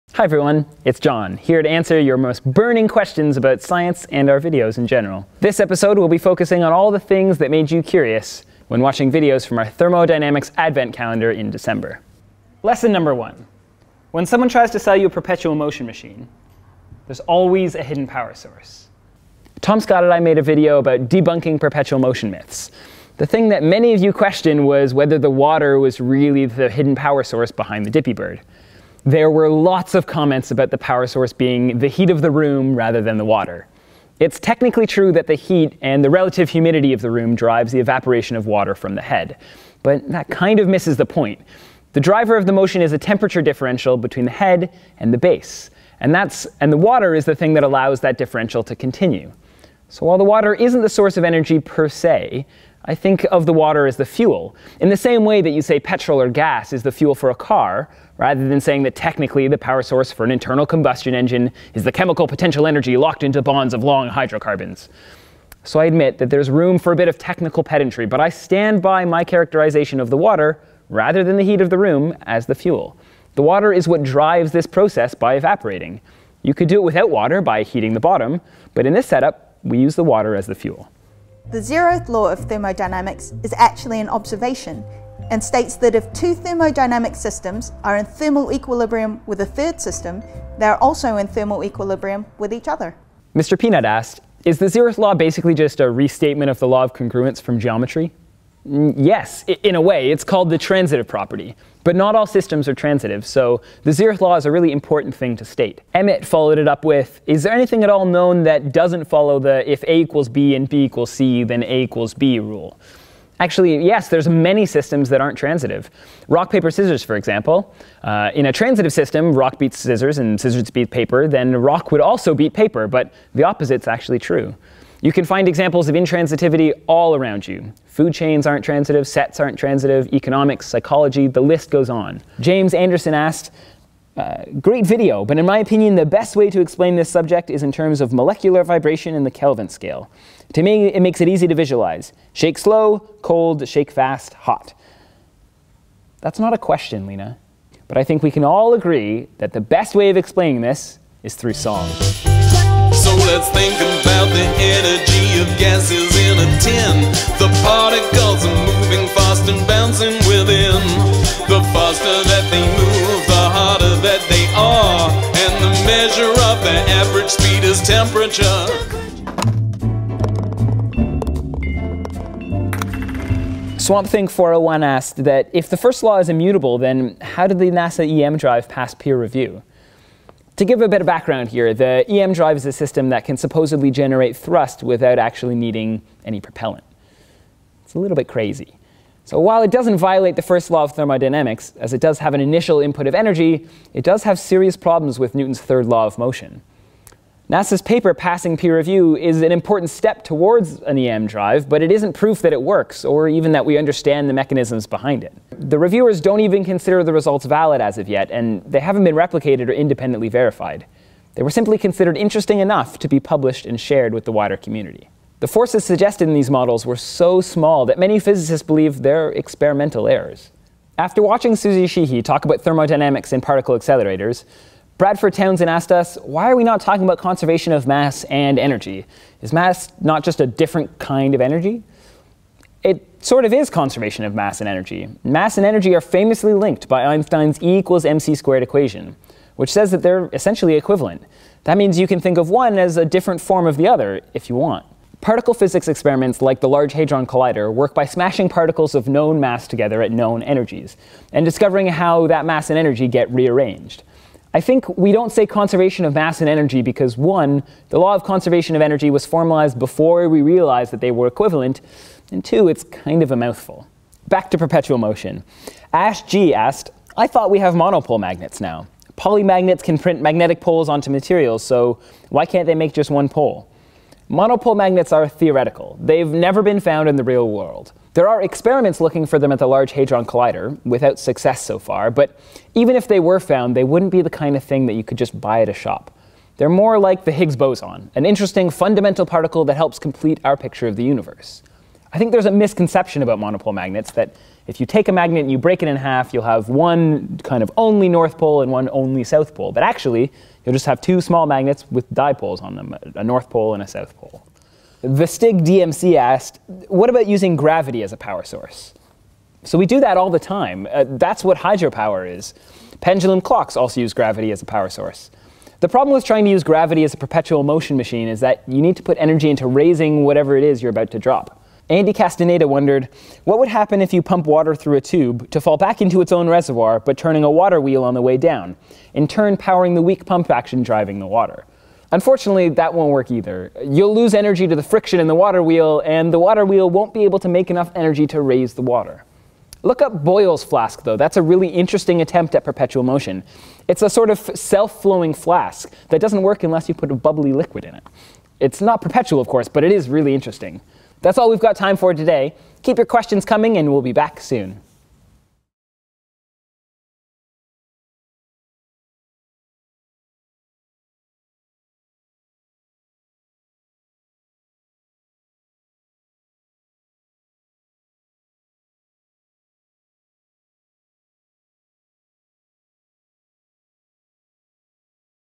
Explaining Thermodynamics - Monthly(ish) Mailbag #3
Jon answers questions from the comments sections, all about thermodynamics.
What actually powers a ‘perpetual’ dippy bird (0:29)? Are there any non-transitive systems (1:33)? How does NASA’S EM drive work if it breaks the first law of thermodynamics (2:45)? Can we make monopole magnets with just one pole (4:20)? Can gravity drive a perpetual motion machine (5:15)?